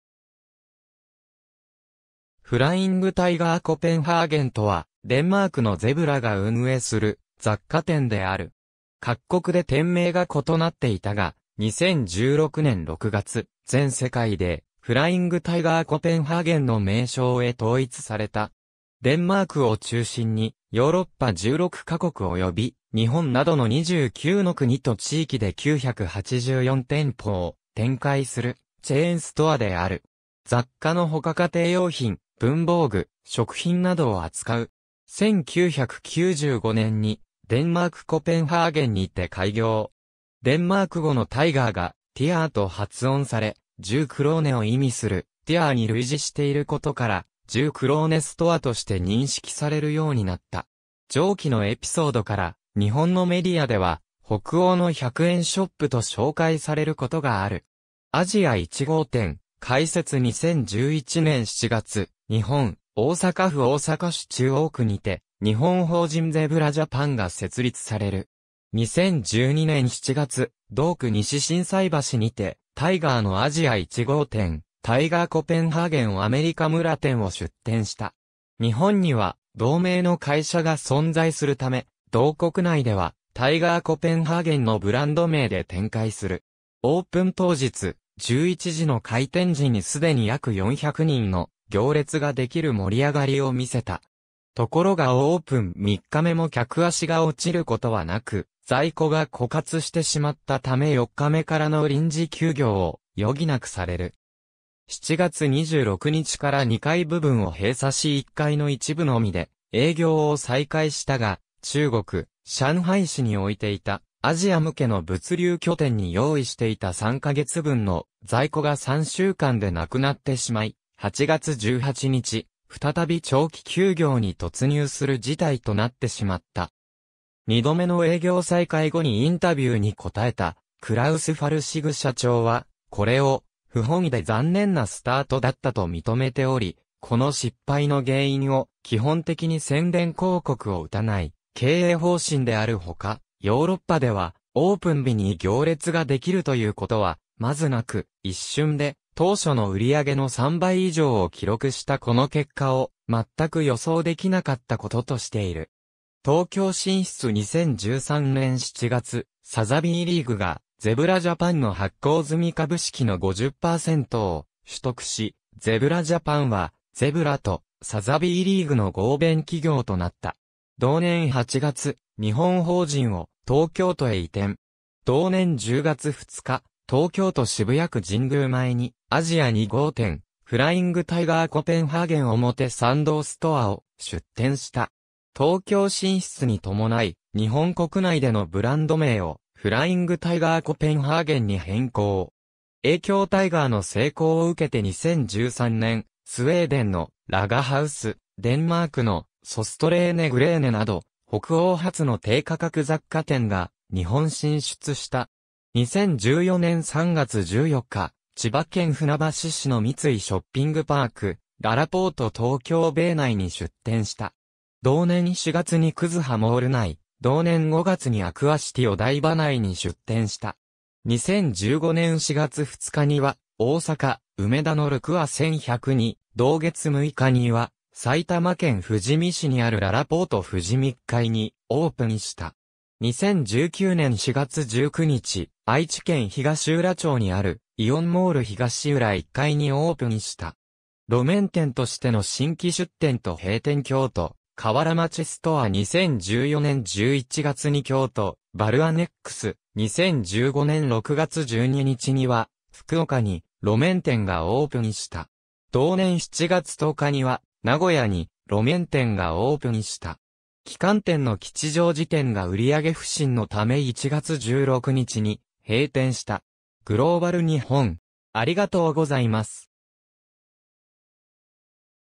フライングタイガーコペンハーゲン
フライングタイガーコペンハーゲン (Flying Tiger Copenhagen)とは、デンマークのゼブラ (ZEBRA A/S) が運営する雑貨店である。
各国で店名が異なっていたが、2016年6月、全世界で「Flying Tiger Copenhagen（フライング タイガー コペンハーゲン）」の名称へ統一された。
デンマークを中心にヨーロッパ16か国（イングランド、ドイツ、ギリシャ、アイスランド、アイルランド、イタリア、ラトビア、リトアニア、オランダ、ノルウェー、ポーランド、スペイン、スコットランド、フィンランド、スウェーデン）および日本などの29の国と地域で984店舗を展開する（2020年3月時点）チェーンストアである。
雑貨のほか家庭用品、文房具、食品などを扱う。
1995年にデンマーク・コペンハーゲンにて開業。
デンマーク語のTIGERが「ティアー(TIGER)」と発音され、10クローネを意味する「ティアー(TIER)」に類似していることから、10クローネストアとして認識されるようになった。
上記のエピソードから日本のメディアでは「北欧の100円ショップ」と紹介されることがある。
アジア1号店開設 2011年7月、日本・大阪府大阪市中央区にて、日本法人ゼブラ ジャパン (Zebra Japan) が設立される。
2012年7月、同区西心斎橋にて、タイガーのアジア1号店「Tiger Copenhagen アメリカ村店」を出店した。
日本には同名（タイガー）の会社が存在するため、同国内では「Tiger Copenhagen（タイガー コペンハーゲン）」のブランド名で展開する。
オープン当日、11時の開店時に既に約400人の行列が出来る盛り上がりを見せた。
ところがオープン3日目も客足が落ちることはなく、在庫が枯渇してしまったため4日目からの臨時休業を余儀なくされる。
7月26日から2階部分を閉鎖し1階の一部のみで営業を再開したが、中国・上海市に置いていたアジア向けの物流拠点に用意していた3か月分の在庫が3週間で無くなってしまい、8月18日、再び長期休業に突入する事態となってしまった。
2度目の営業再開後にインタビューに答えたクラウス・ファルシグ社長は、これを「不本意で残念なスタート」だったと認めており、この失敗の原因を、基本的に宣伝広告を打たない経営方針であるほか、ヨーロッパではオープン日に行列が出来るということはまずなく、一瞬で当初の売上の3倍以上を記録したこの結果をまったく予想出来なかったこととしている。
東京進出 2013年7月、サザビーリーグがゼブラ ジャパンの発行済株式の50%を取得し、ゼブラ ジャパンはゼブラとサザビーリーグの合弁企業となった。
同年8月、日本法人を東京都へ移転。
同年10月2日、東京都渋谷区神宮前にアジア2号店「Flying Tiger Copenhagen 表参道ストア」を出店した。
東京進出に伴い、日本国内でのブランド名を「Flying Tiger Copenhagen（フライング タイガー コペンハーゲン）」に変更。
影響 タイガーの成功を受けて2013年、スウェーデンの『ラガハウス』、デンマークの『ソストレーネ・グレーネ』など北欧発の低価格雑貨店が日本進出した。
2014年3月14日、千葉県船橋市の三井ショッピングパーク・ららぽーとTOKYO-BAY内に出店した。
同年4月にKUZUHA MALL内（大阪府枚方市）、同年5月にアクアシティお台場内（東京都港区台場）に出店した。
2015年4月2日には大阪・梅田のルクア1100に、同月6日には埼玉県富士見市にあるららぽーと富士見1Fにオープンした。
2019年4月19日、愛知県東浦町にあるイオンモール東浦1Fにオープンした。
路面店としての新規出店と閉店 京都河原町ストア 2014年11月に京都BAL ANNEX、2015年6月12日には、福岡（天神）に路面店がオープンした（2020年3月移転のため休店）。
同年7月10日には、名古屋（栄）に路面店がオープンした。
旗艦店の吉祥寺店が売上不振の為1月16日に閉店した。
グローバル 日本